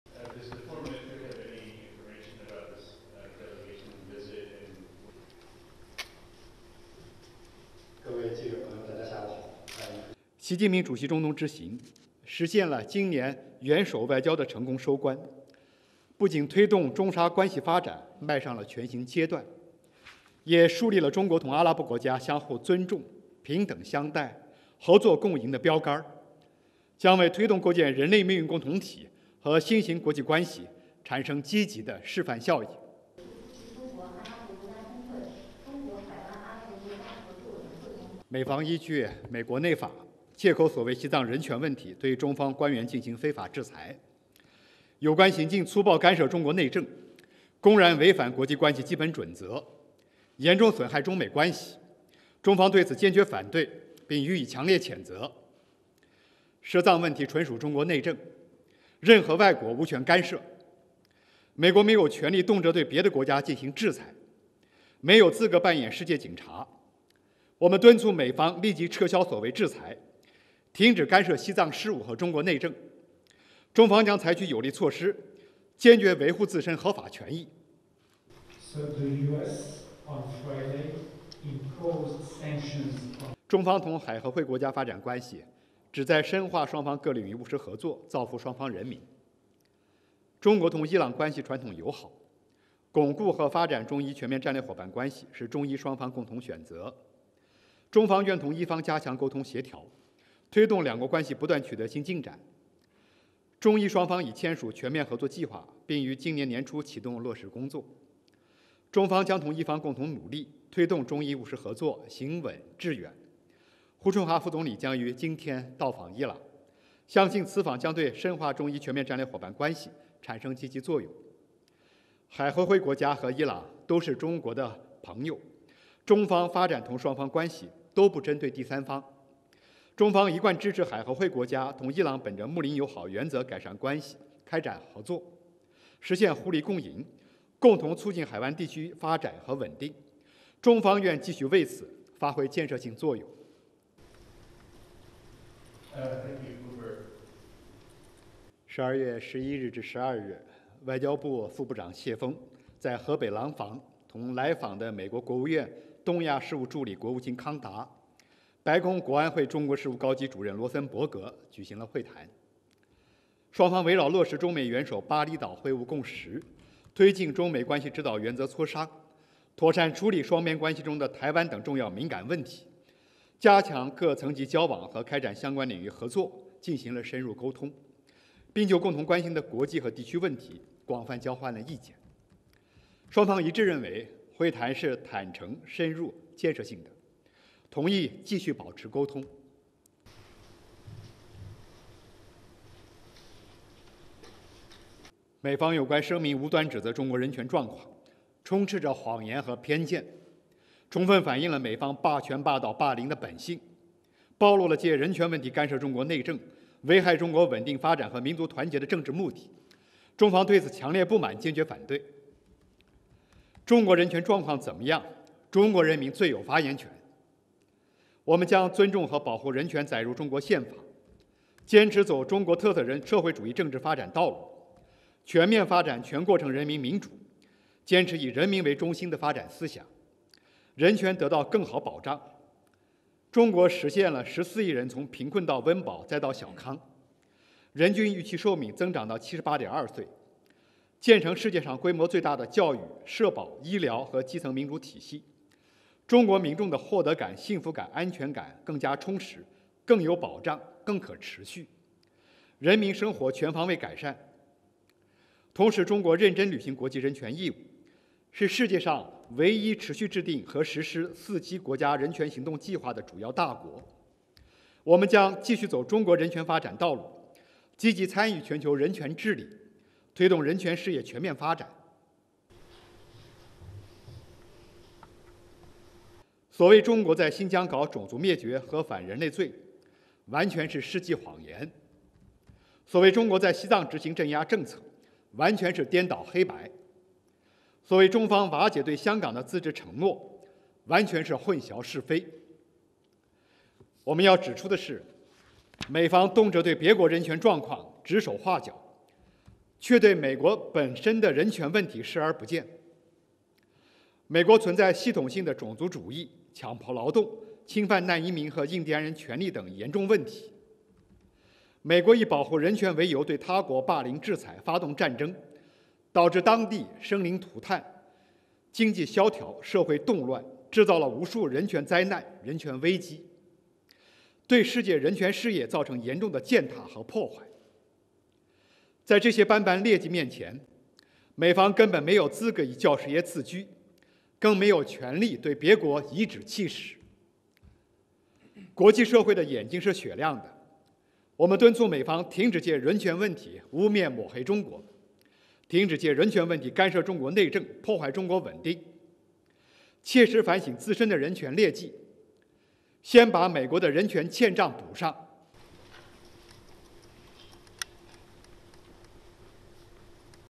MOFA client briefing
ASSOCIATED PRESS
Beijing -  12 December 2022
1. President Xi Jinping concluded his visit to Saudi Arabia, where he attended two regional summits with Middle East countries. What’s the significance of his trip?

President Xi Jinping's trip to the Middle East is a successful conclusion to this year's head of state diplomacy, which not only promotes the development of China-Arab relations to a new stage, but also sets a benchmark of mutual respect, equal treatment and win-win cooperation between China and Arab countries. It will have a positive demonstration effect for promoting the building of a community of shared destiny and a new type of international relations.

2. The United States announced last week that it had imposed sanctions on two Chinese officials for alleged human rights abuses in Tibet. What’s your comment?

The U.S. illegally imposed sanctions on Chinese officials under the pretext of the so-called human rights issues in Tibet in accordance with its domestic law. This grossly interferes in China's internal affairs, blatantly violates the basic norms of international relations and constitutes serious damages to China-U.S. relations.
Tibet-related issues are purely internal affairs of China, and no foreign country has the right to interfere. The U.S. is in no position to willfully sanction other countries or act as a world policeman. We urge the U.S. to immediately withdraw the so-called sanctions and stop interfering in Tibetan affairs and China's internal affairs. China will take strong measures to resolutely safeguard its legitimate rights and interests.

3. A few days ago, the China-Gulf Cooperation Council (GCC) summit was held in Riyadh, and the leaders reached an important consensus on consolidating political mutual trust and strengthening cooperation in various fields between the two sides. At the same time, there are comments that China will not neglect its relations with Iran while developing relations with the GCC countries. We also note that Vice Premier Hu Chunhua will visit Iran soon. What is China's comment on this?

China's relations with the GCC countries are aimed at deepening practical cooperation in various fields for the benefit of the people of both sides. China and Iran have a tradition of friendly relations, and it is a common choice for both sides to consolidate and develop the China-Iran comprehensive strategic partnership. China is willing to strengthen communication and coordination with the Iranian side to promote new progress in the relations between the two countries. China and Iran have signed a comprehensive cooperation plan and will start putting it into practice at the beginning of this year. China will work with the Iranian side to promote pragmatic cooperation between China and Iran in a steady and far-reaching manner. Vice Premier Hu Chunhua will visit Iran today and we believe that this visit will have a positive effect on deepening China-Iran comprehensive strategic partnership.
Both the GCC countries and Iran are friends of China, and China's development of relations with both sides is not targeted at third parties. China has always supported the GCC countries to improve relations and cooperation with Iran in accordance with the principle of good neighborliness and friendship, so as to achieve mutual benefits and win-win results, and jointly promote the development and stability of the Gulf region. China is willing to continue to play a constructive role to this end.

4. Two officials from the U.S. State Department have visited China, can you share more information?